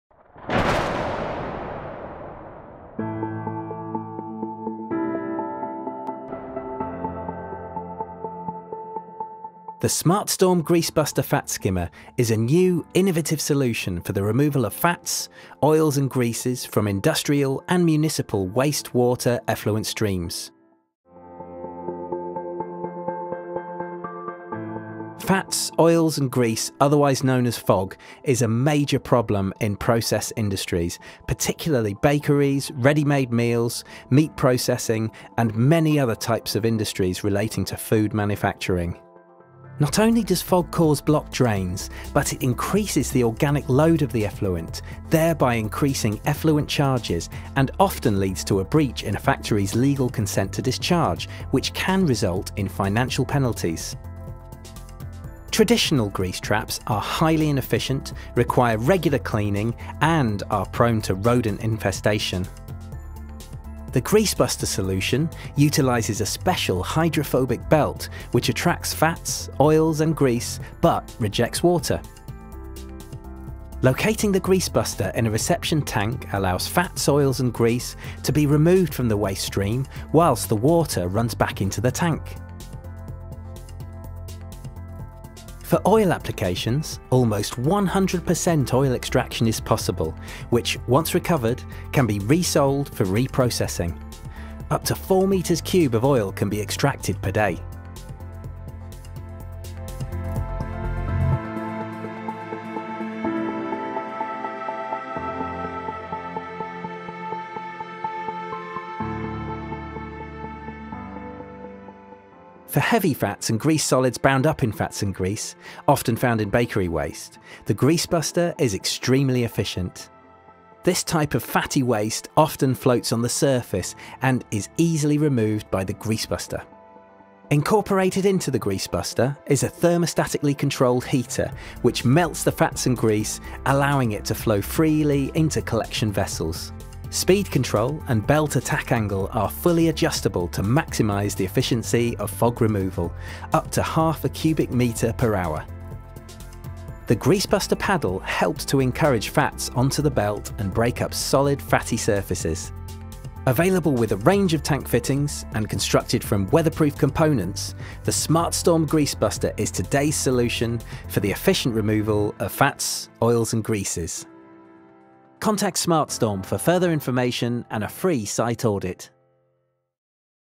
Grease Buster Full Video 2022 - Removal of Fats, Oils and Greases.
The Greasebuster® is an innovative solution that removes fats, oils and grease from wastewater tanks and industrial effluent systems.  The Greasebuster® oil skimmer uses a specially treated hydrophobic belt which attracts non-polar liquids such as fats, oils and greases whilst rejecting water. The Greasebuster® can be installed on a reception tank with the belt  continuously rotating just below the water level removing any surface materials.  The free phase surface FOG's are recovered whilst the flow of waste water returns to the source tank.     This is an extremely popular solution for the food industry as it is very robust and reliable.  The removal of very solid FOG's sets this product apart from other oil skimmers.  Ideal for export markets due to the simple service tasks required to run it efficiently.